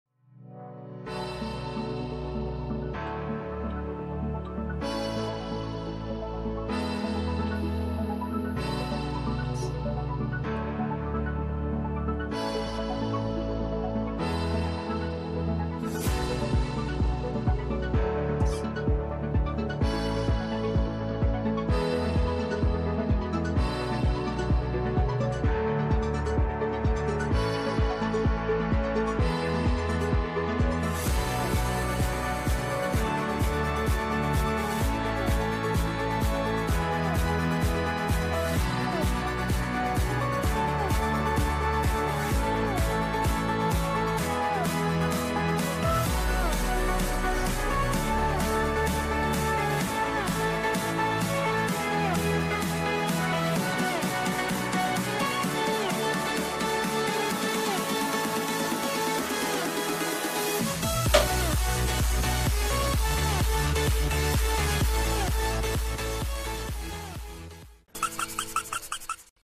Robotic Arm automatically removing Print Jobs from a 3D Printer
3D Printed Robot Arm taking 3D Printed Robot Arm Parts off a 3D Printer.